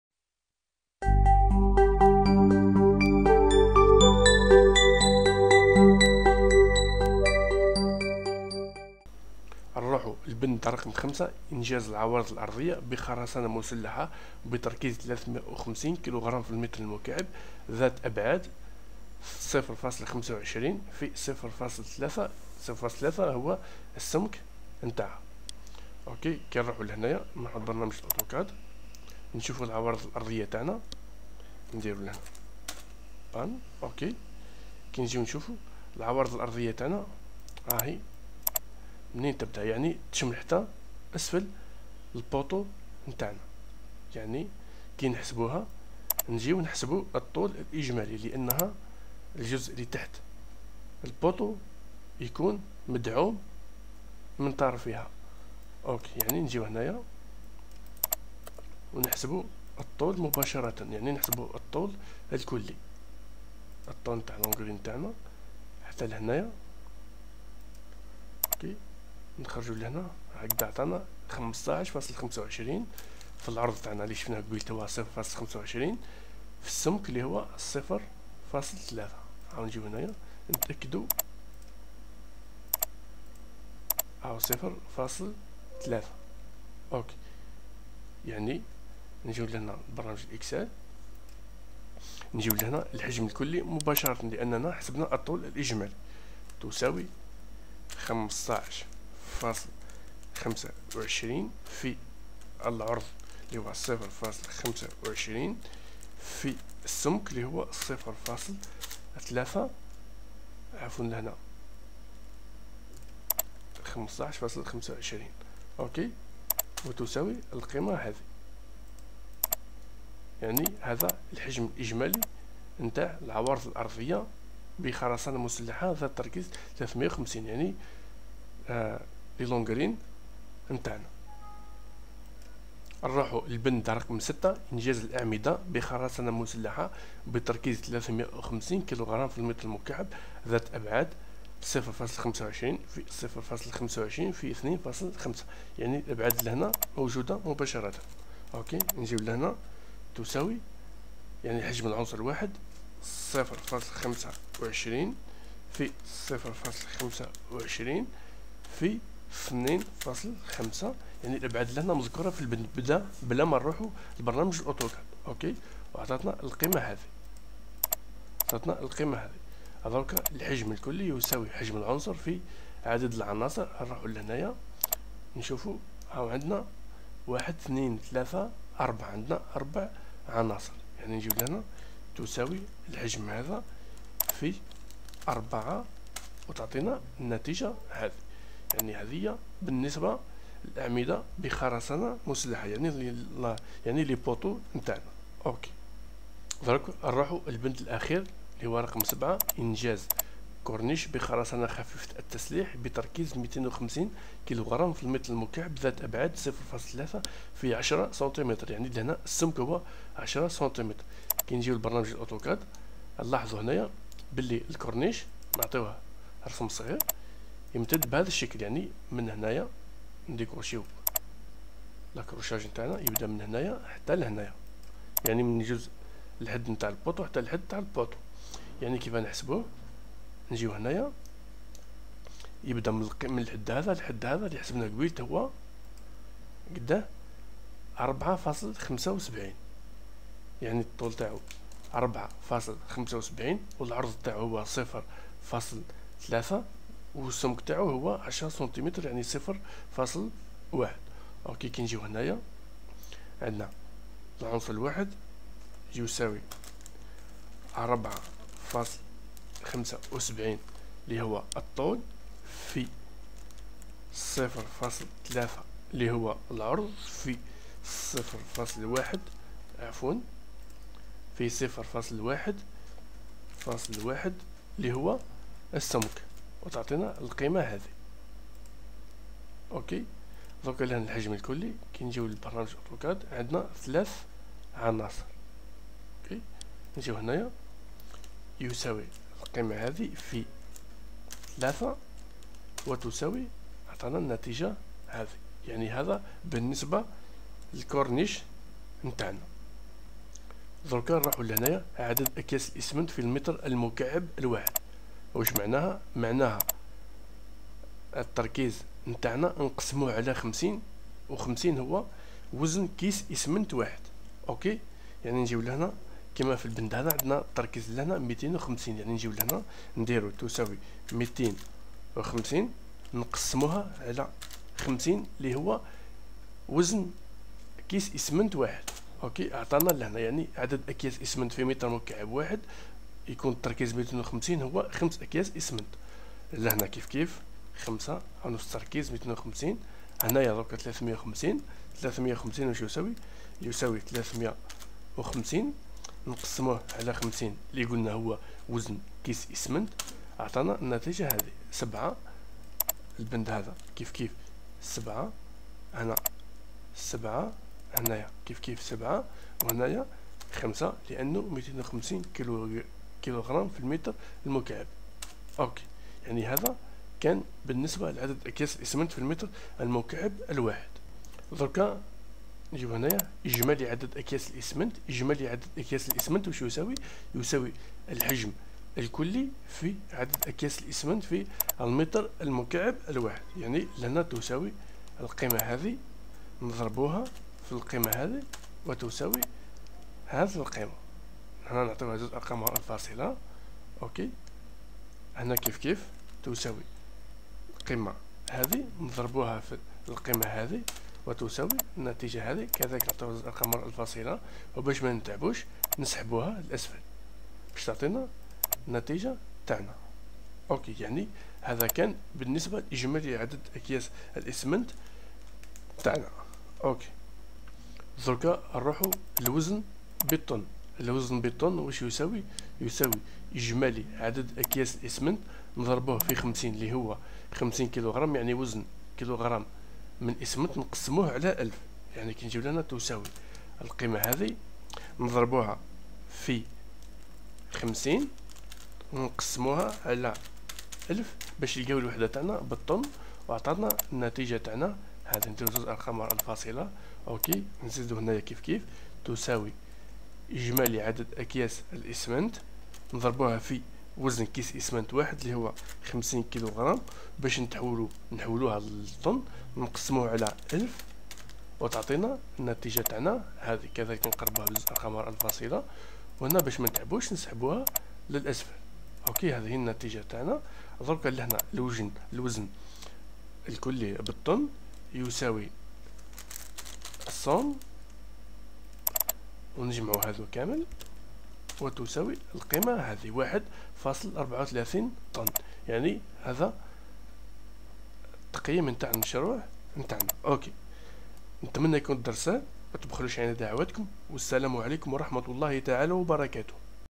الجزء الثاني: شرح تقدير كميات الاسمنت (شهادة احتياج الاسمنت) بالأكياس و الطن في المشاريع
السلام عليكم
 (Attestation des besoins en ciment)